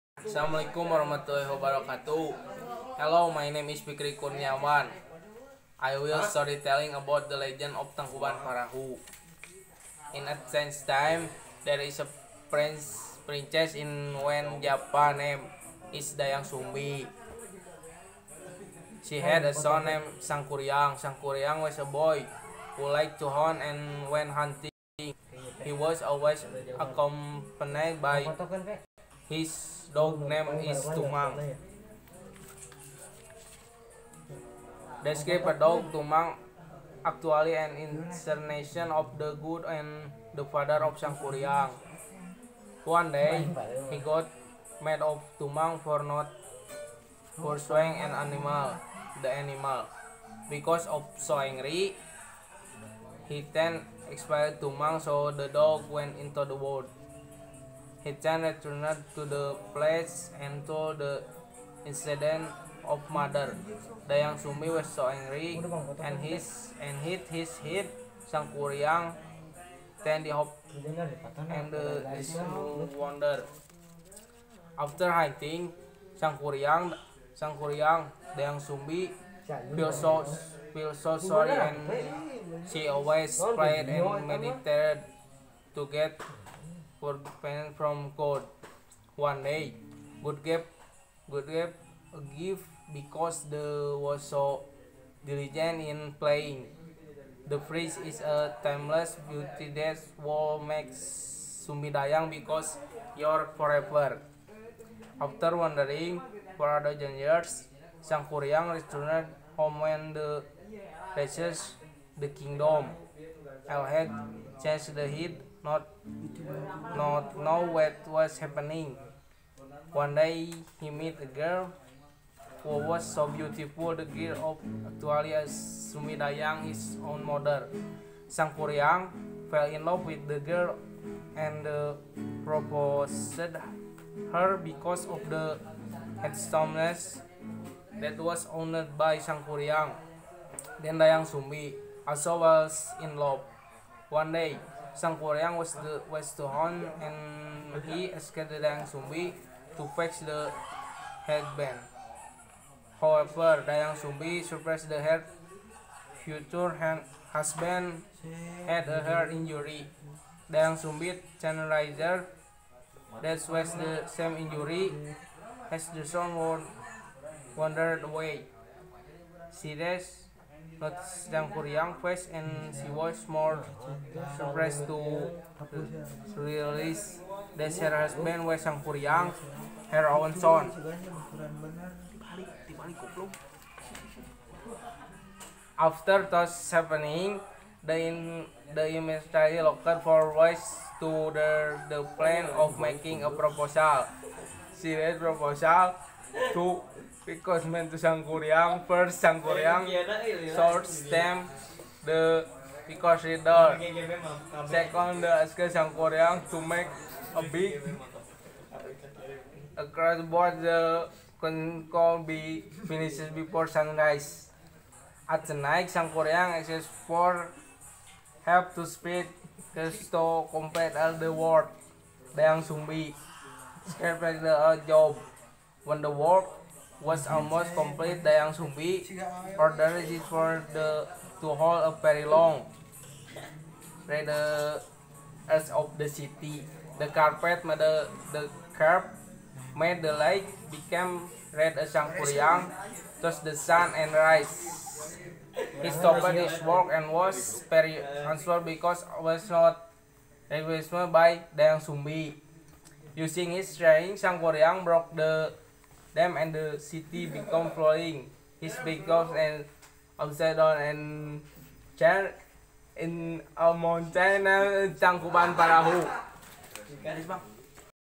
Storytelling the legend about "tangkuban perahu" by.Fikri kurniawan
Mohon maaf jika ada kesalahan karna manusia tidak luput dari kesalahan  🙏